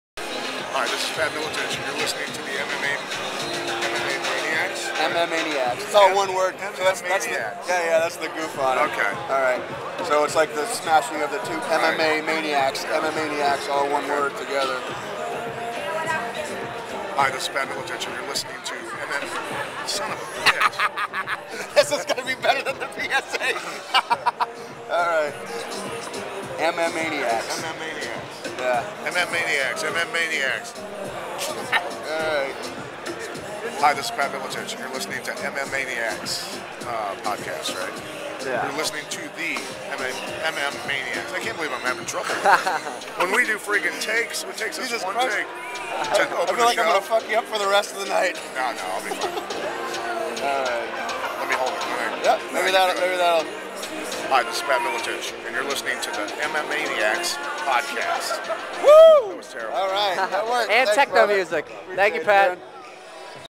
Pat Miletich (and techno music) 'Making of a Podcast Stinger' MMAniacs Round 3
Pat Miletich (and techno music) and the behind the scenes "making of recording a podcast stinger."  More details on MMAniacs Podcast Round 3 (coming on Monday)